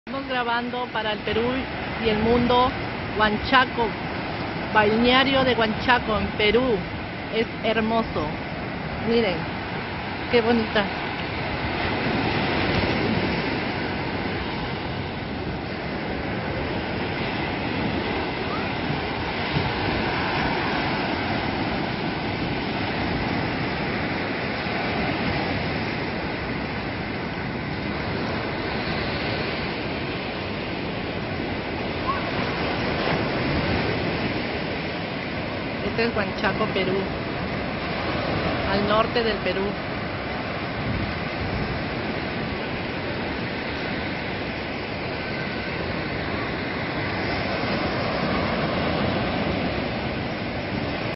Hermosa playa huanchaco trujillo peru veraneaga
Donde hay los barcos de totora de paseo rica comida de pescado y msriscos lindo lugar todo el año viajen